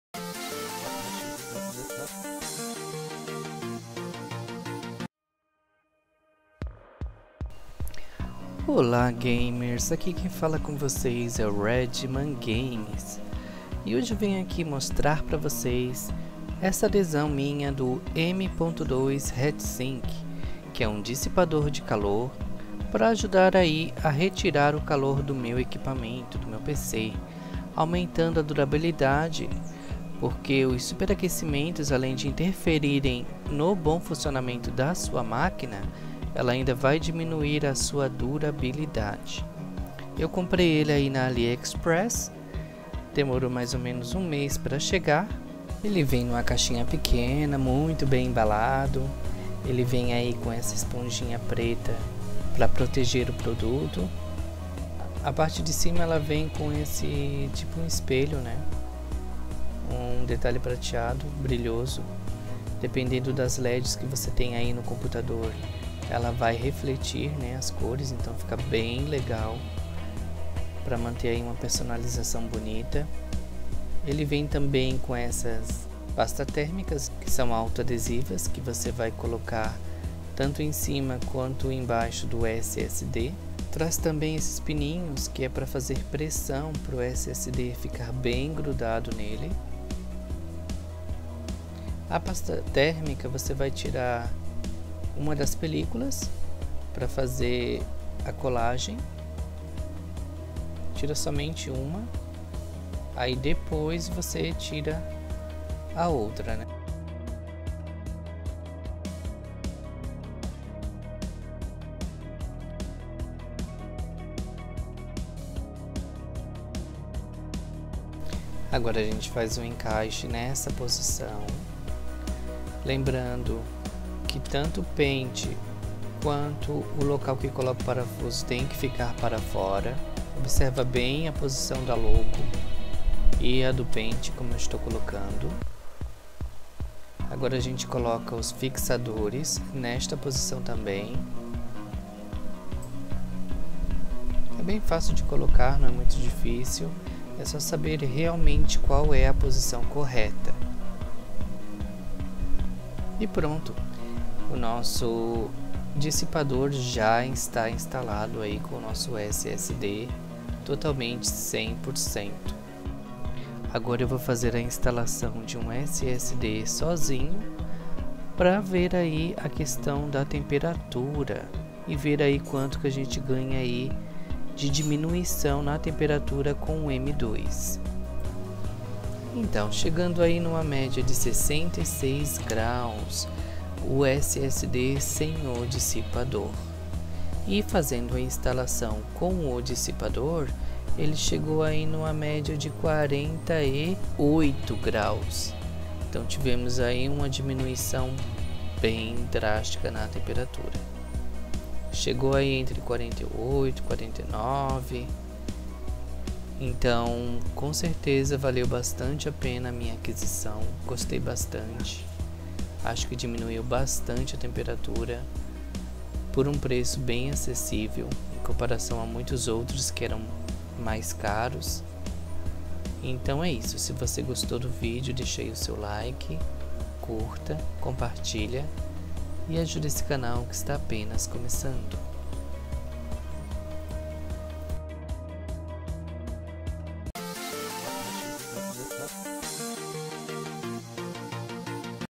M.2 Heatsink Snowman Instalação e teste temperatura dissipador de SSD M.2
M.2 Heatsink Snowman Instalação e teste de temperatura do dissipador de SSD M.2